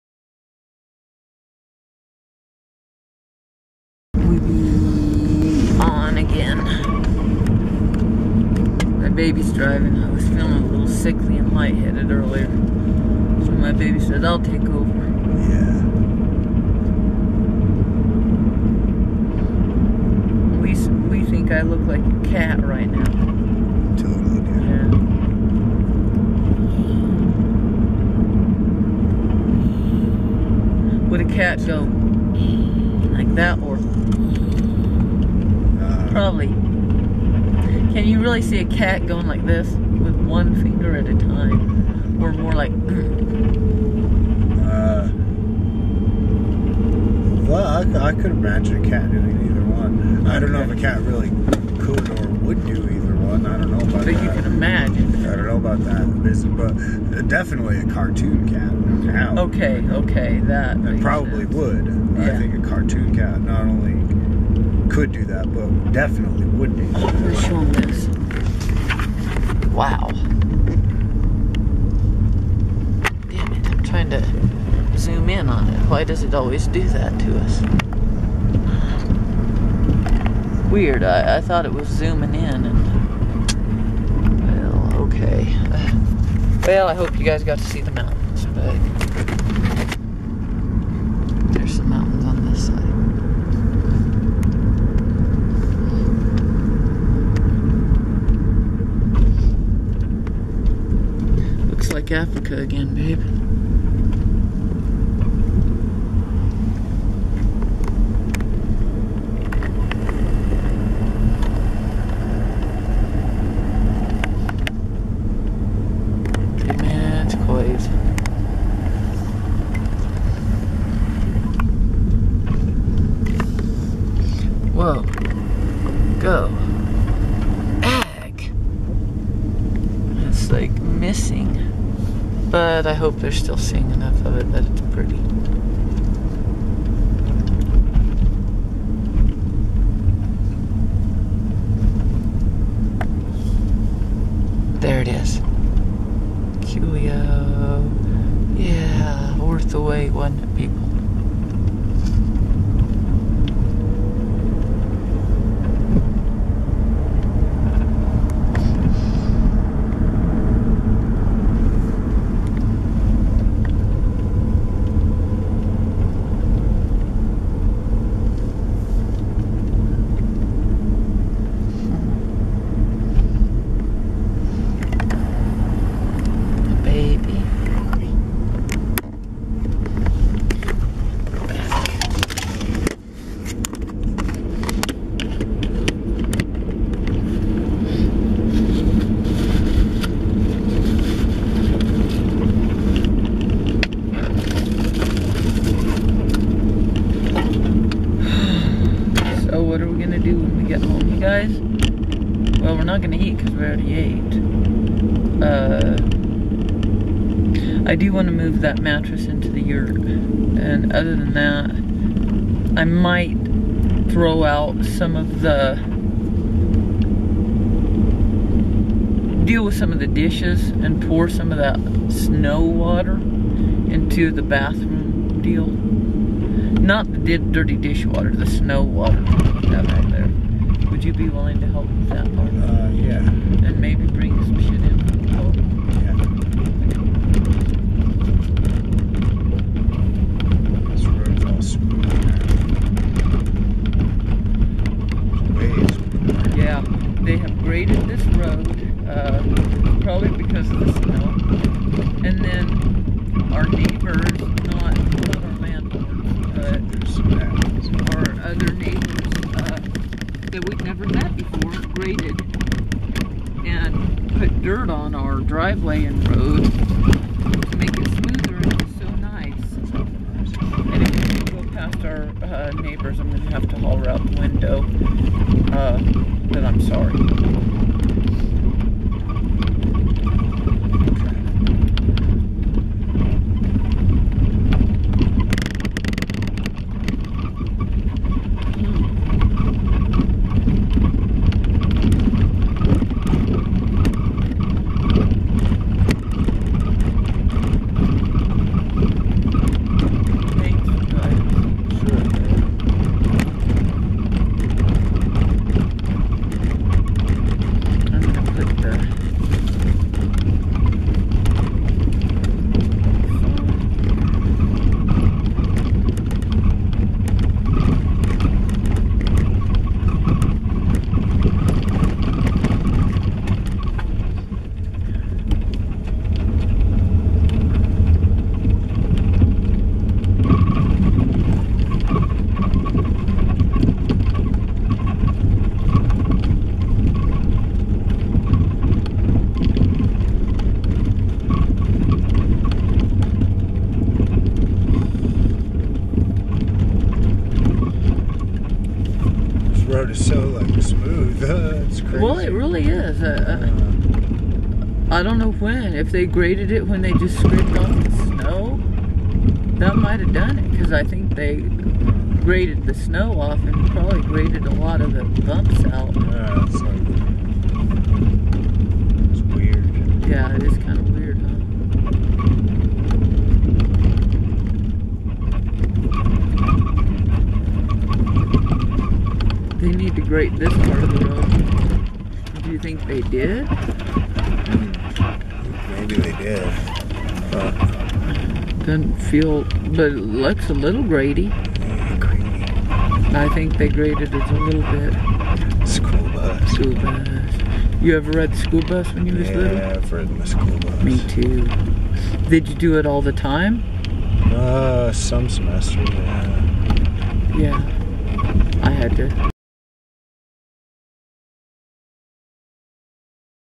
Sickly & Lightheaded
. . . the twins on a return trip to the mountains when they still lived there.